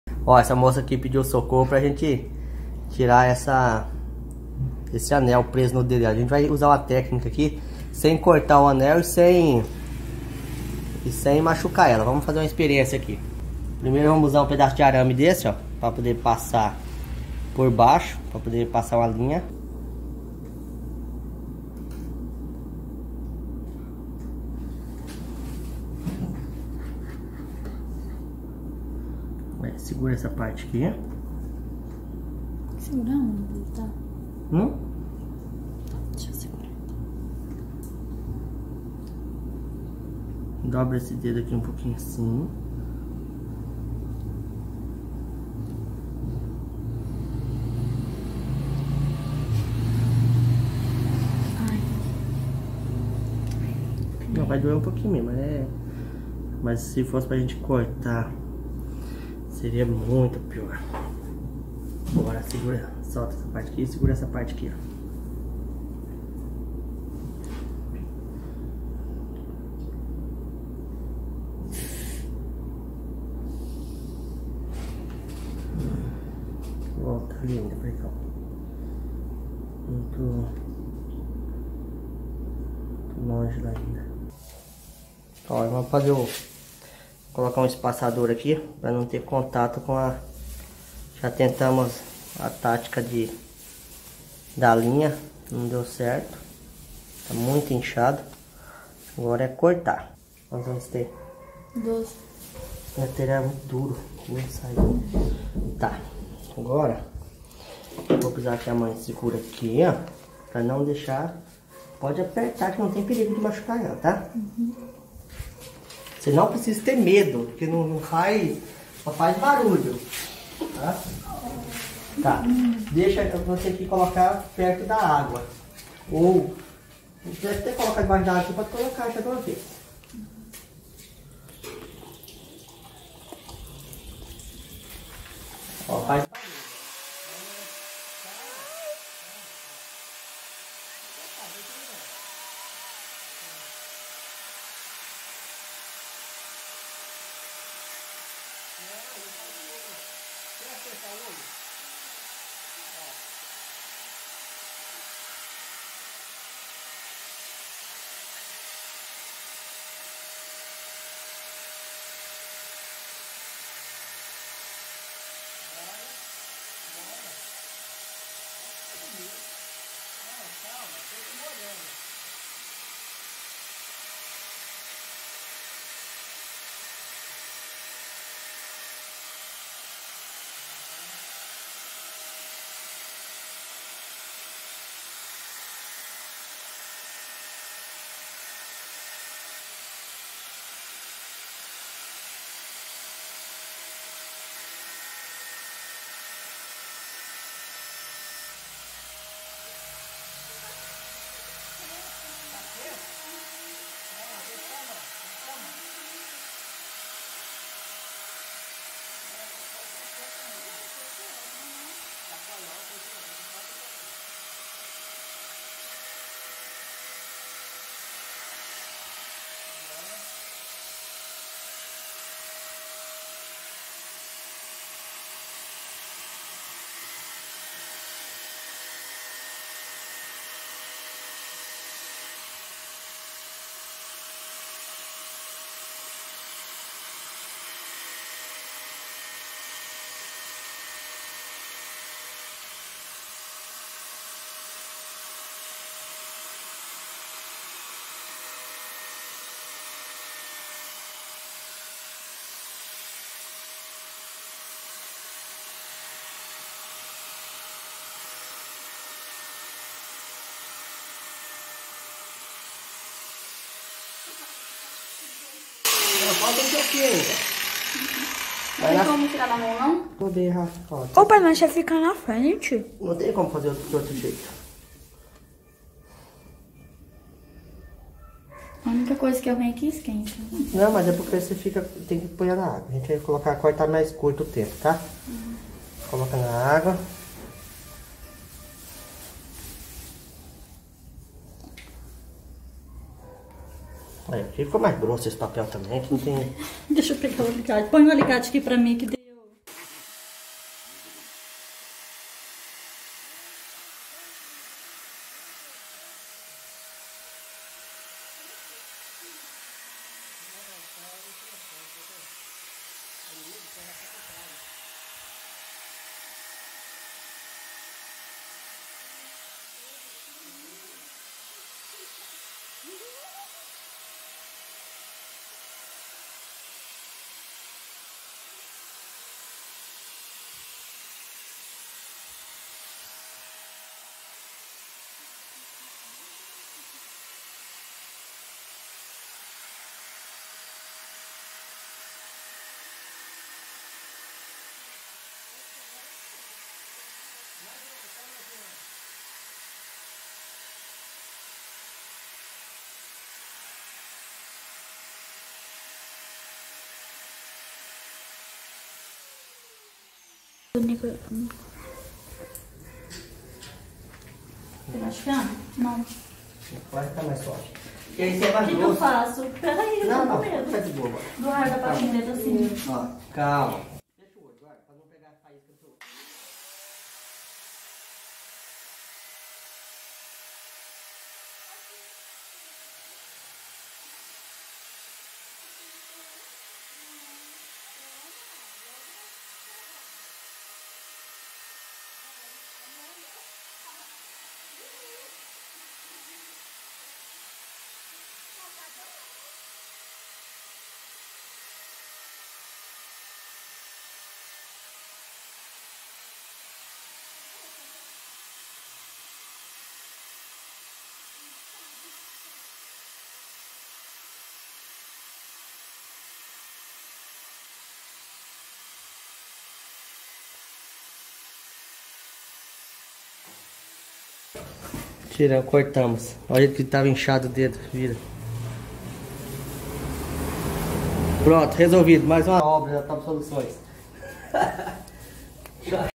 TIRANDO O ANEL DO DEDO INCHADO
uma familia me procurou para cortar um anel com uma serrinha, tentamos outros meios mas foi na serra eletrica que conseguimos resolver Brasnorte mato grosso clemilson doido